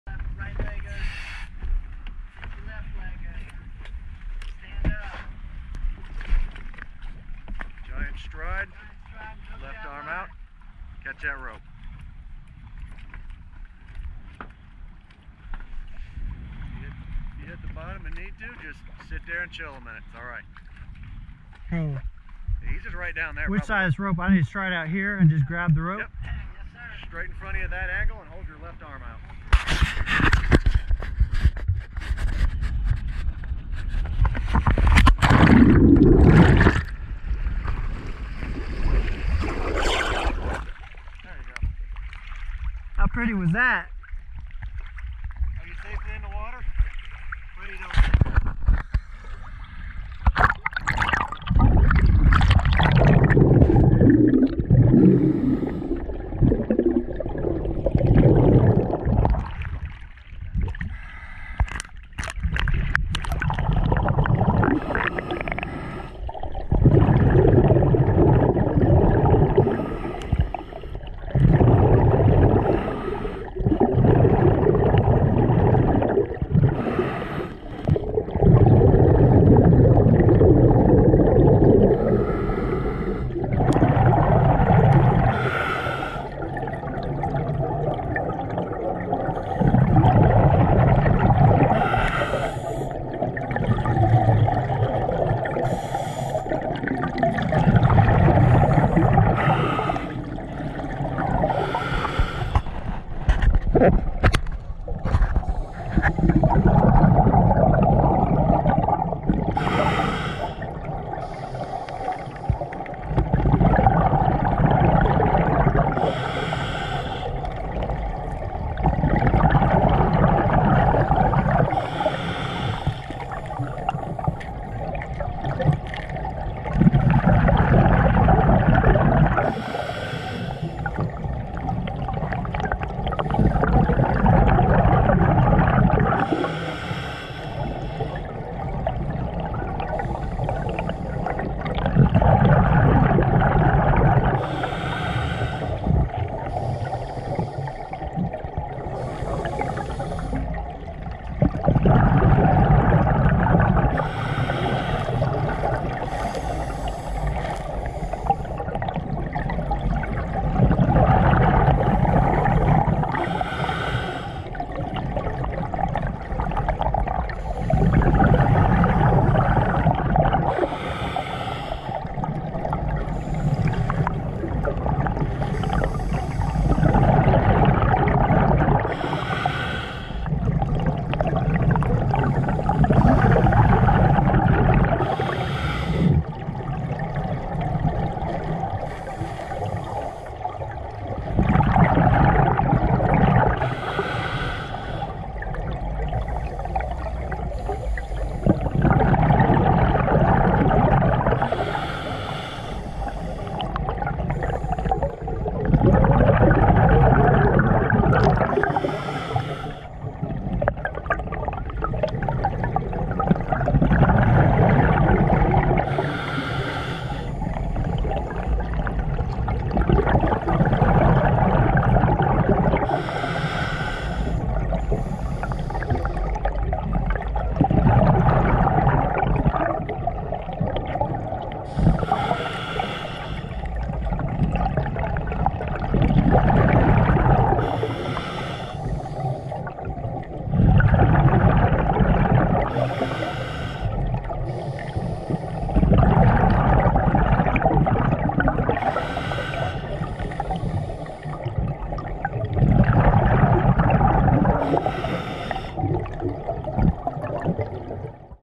Black Water Diving
This video demonstrates a condensed version of the SCUBA experience "from boat to river-bottom" searching for shark teeth in the "black water" of Kingsland, GA.